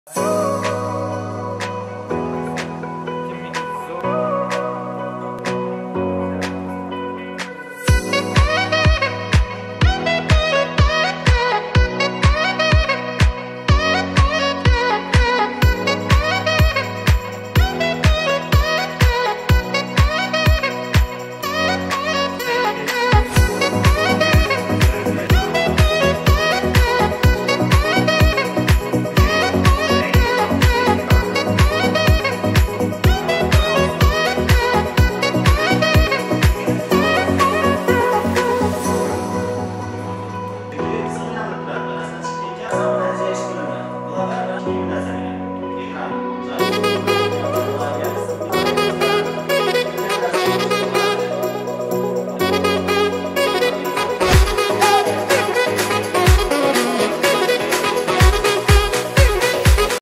🔥 "DATA" innovatsion o'quv markazi dars jarayonlaridan videolavha
Videoda siz quyidagi kurs dars jarayonlarini ko'rishingiz mumkin:
🔹 Operatorlikka asoslangan video montaj;
🔹 Buxgalteriya noldan balansgacha;
🔹 PHP'ga asoslangan web dasturlash.

😎 Agar siz ham kurslarimizda o'qishni istasangiz hoziroq administratorlarimiz bilan bog'lanib yoki online anketa to'ldirib o'z joyingizni band qiling.